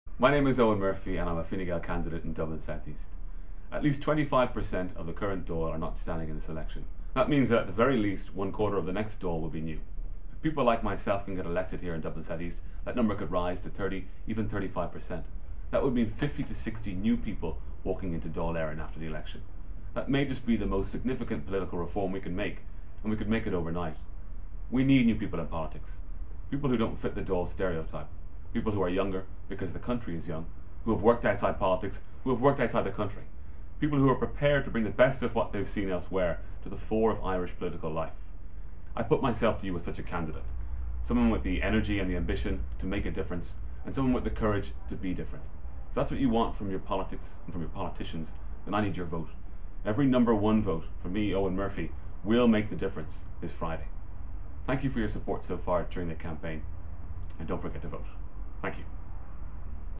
Vote Murphy #1 on Friday
Help elect Eoghan Murphy to the 31st Dáil Eireann on Friday by giving him your number one preference.